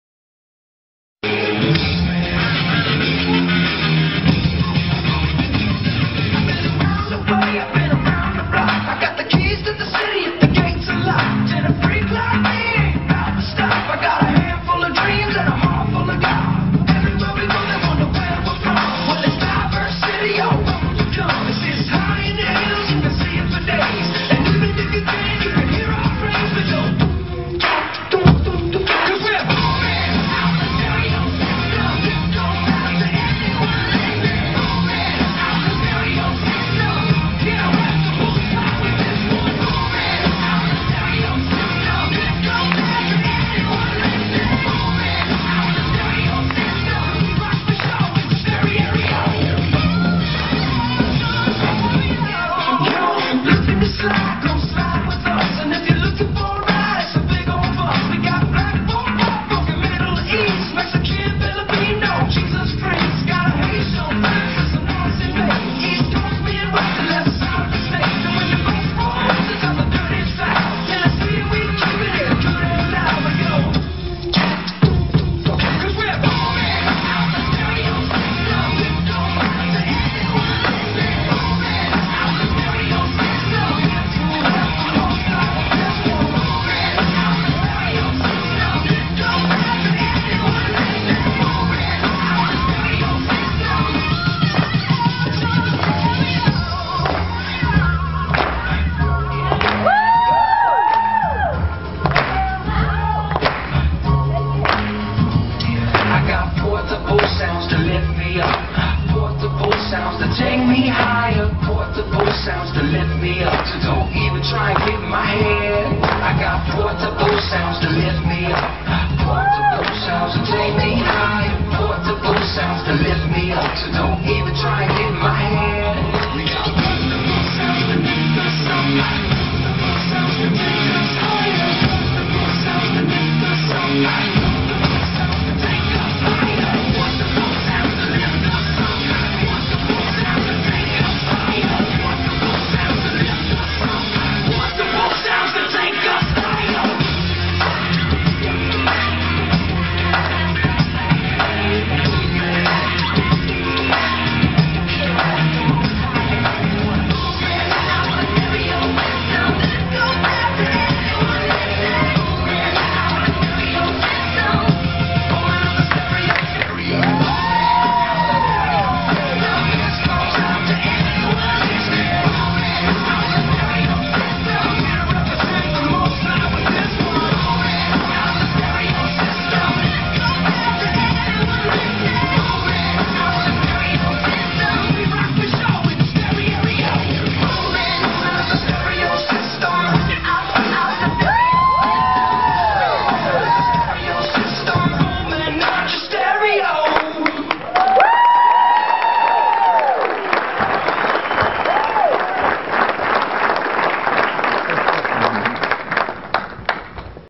SPIN Boomin Blacklight Dance
Northview Community Church. SPIN...
Praise team for 1st - 6th grade
they choreographed it all themselves...incredible and talented group
of teens...